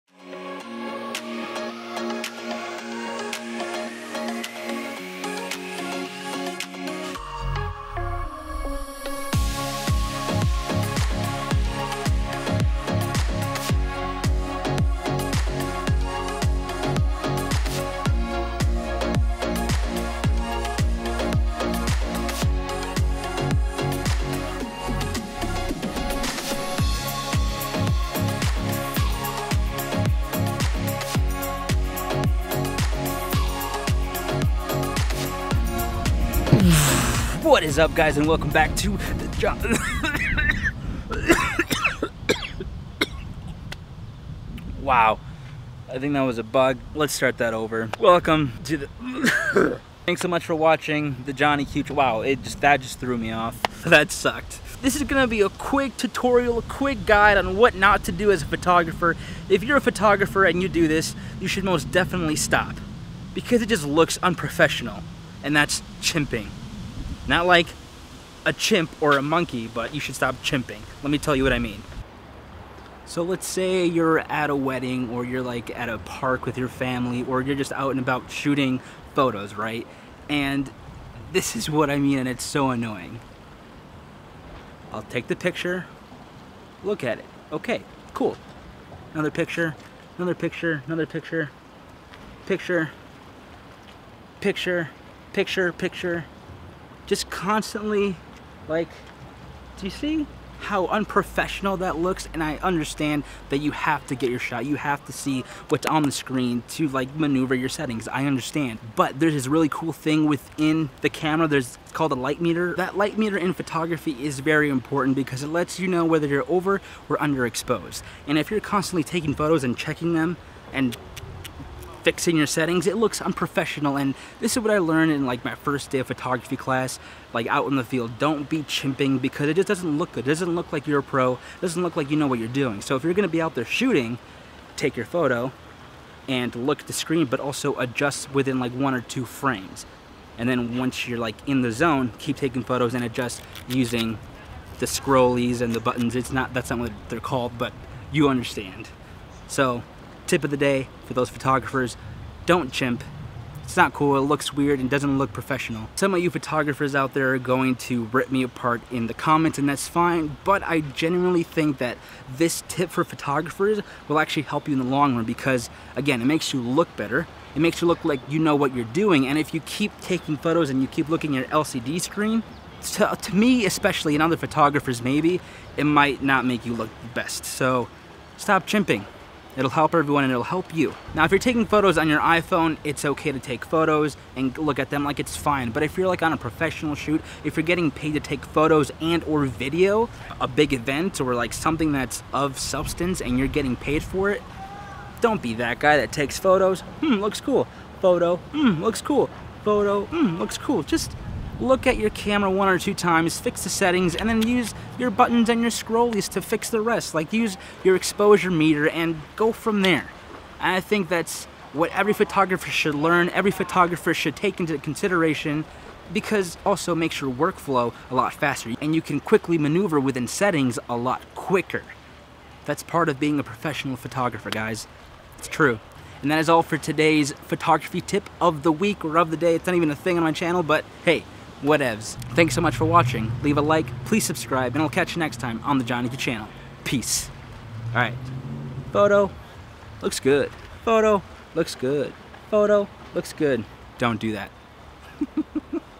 REAL Photographers Don't Do This - STOP THE CHIMPING
If you want to be a professional photographer then you need to stop doing this. It's called "chimping". Have you ever seen that one photographer that takes a picture and immediately looks down at his or her screen to see if they got the shot? But then proceeds to do so after EVERY SHOT!? Yeah, that's chimping and it's annoying to see. As a professional photographer, you need to know how to handle your camera settings in ANY scenario. It's about getting the shot and focused on the screen. Sure, you can look at your screen every now and then to make sure your exposure is correct as well as focus but don't look at every shot you take. Be a professional and engage with your subject more than your LCD screen.